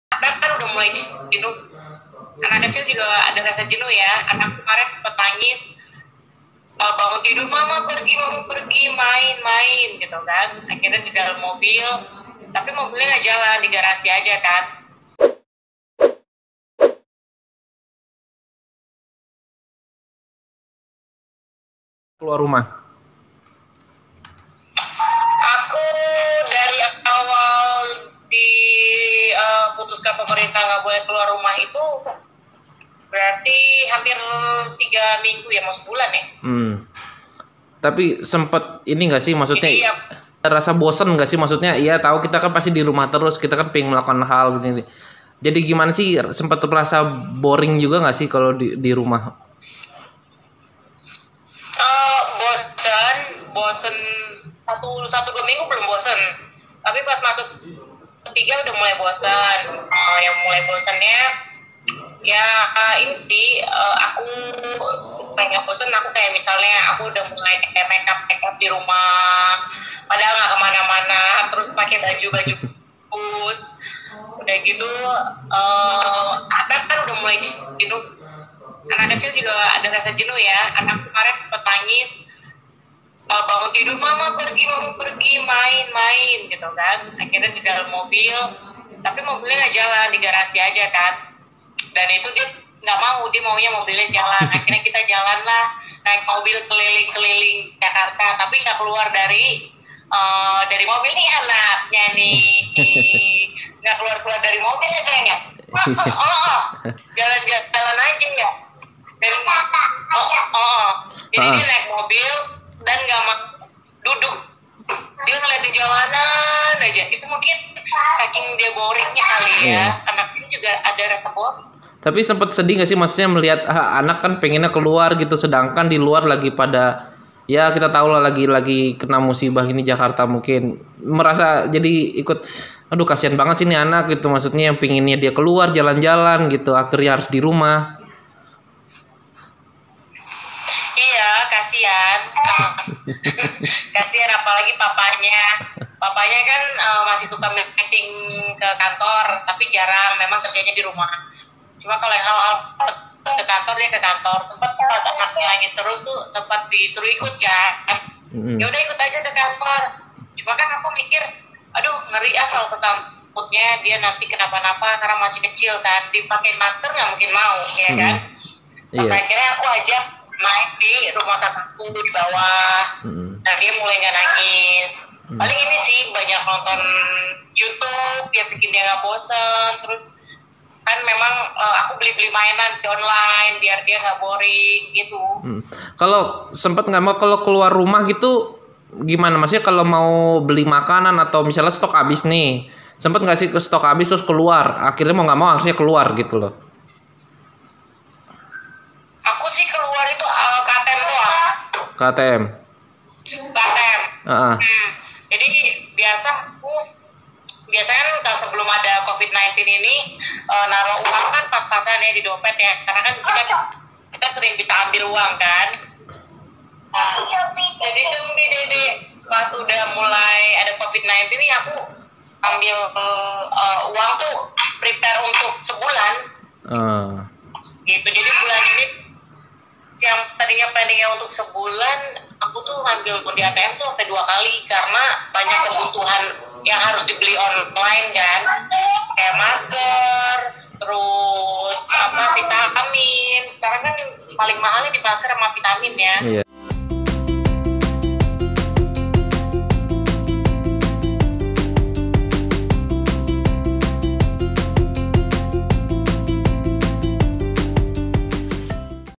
Jenny Cortez Bagikan Tips, Cara Hilangkan Kejenuhan Buah Hati Saat Berada di Rumah Aja
Kondisi sekarang membuat Jenny Cortez lebih banyak melakukan aktivitas di rumah. Buah hati Jenny pun kerap merasa jenuh atau bosan saat hanya berada di rumah saja. Kepada Beepdo.com dalam wawancara Kamis (16/4), Jenny memberikan tips bagaimana cara menghilangkan kejenuhan putrinya tersebut. (Gede/Beepdo.com)